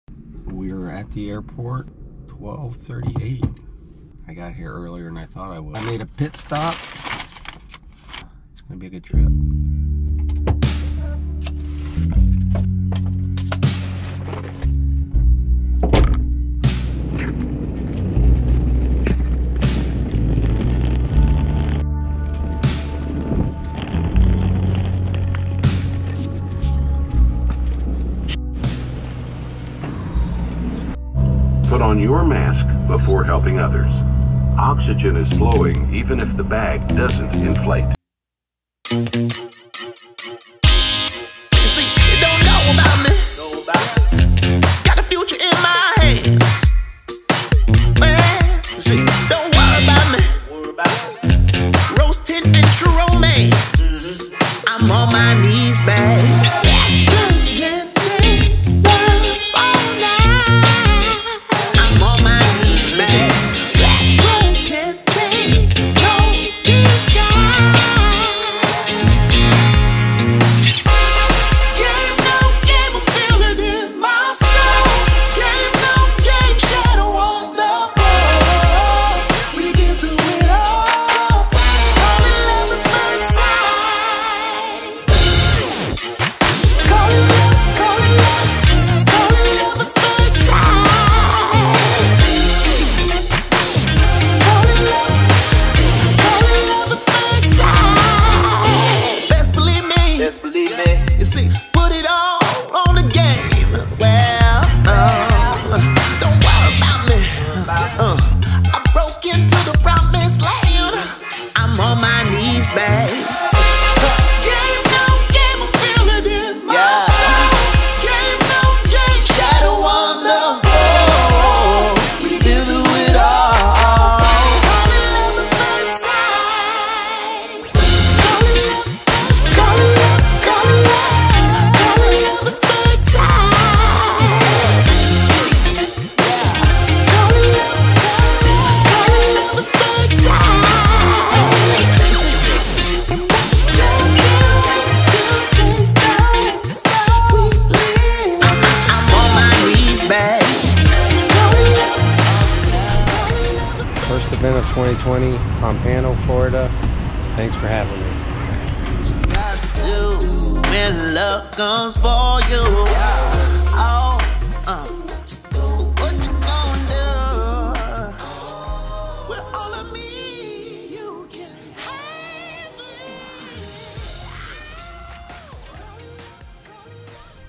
How to create a Street Painting
I'm Jeff. I am a featured chalk artist at many of these chalk festivals around the country. This year I was invited to be a part of an event in Pompano Beach Florida. ArtLit 2020. The theme of the event was "Remembering the Past, Envisioning the Future". This is why I chose to do a portrait of Steve Jobs.

I edit with Adobe Creative Cloud Products Adobe Premiere Pro, Photoshop etc.